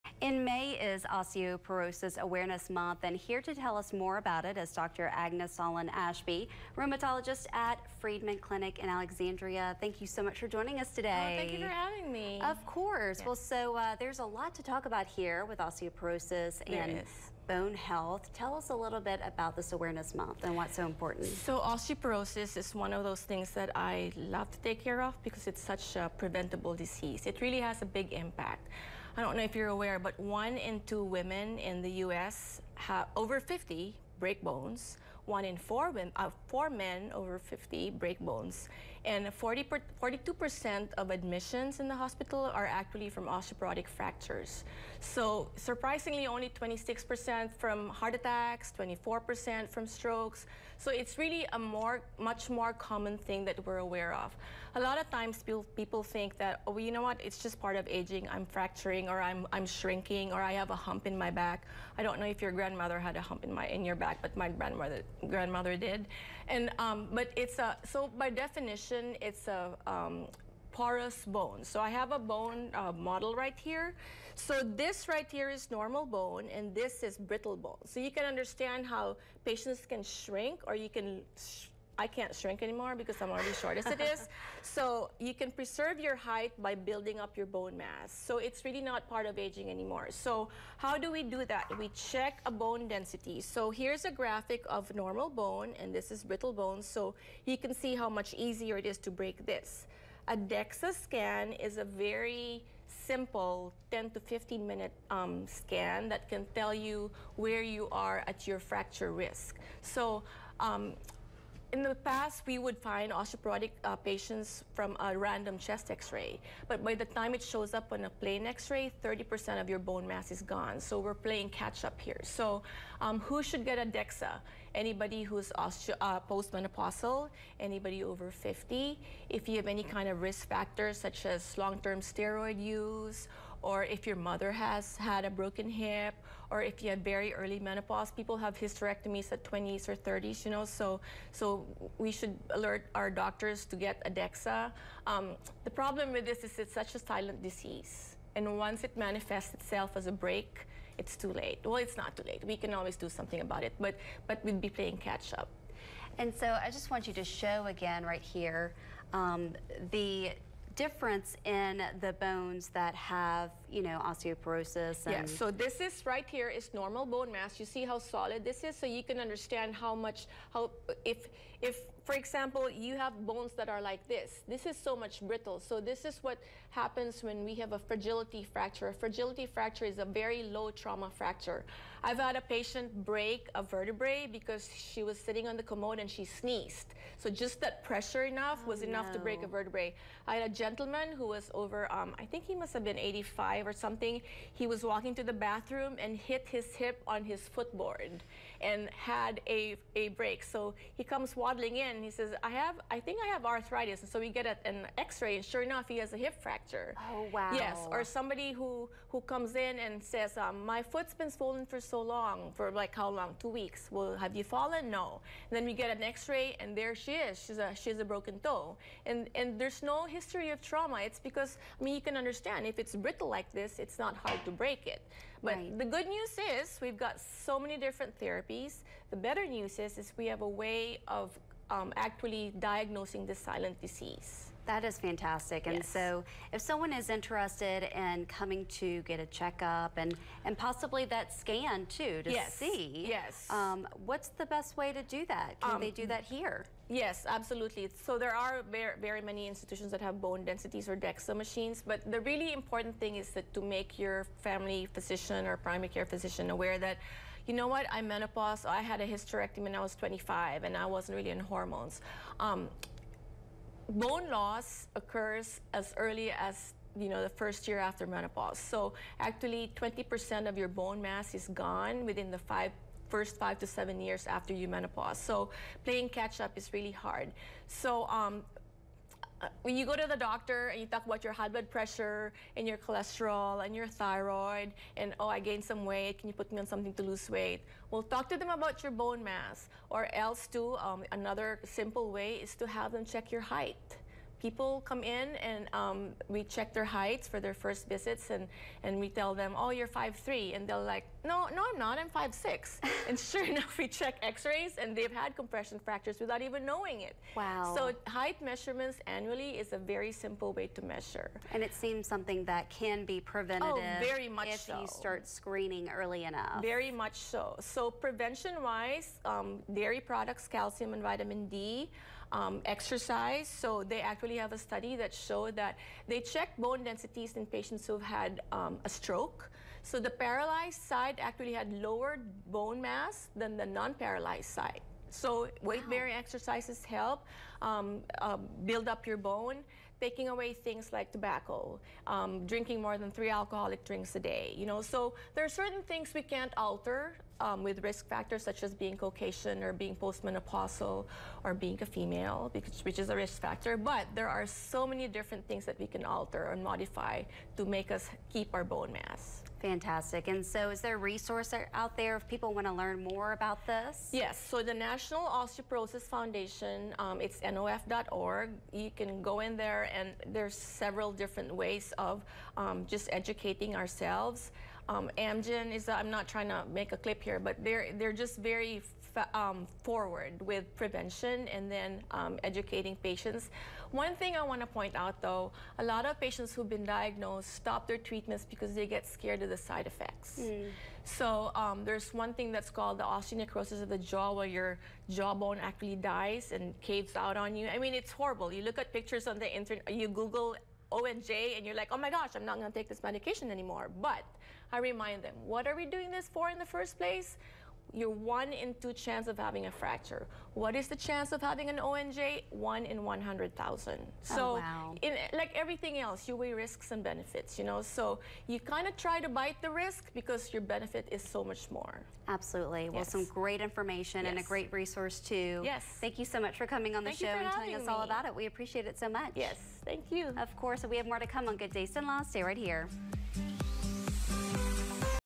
Osteoporosis Awareness Month
Agnes Solon-Ashby talks about Osteoporosis Awareness Month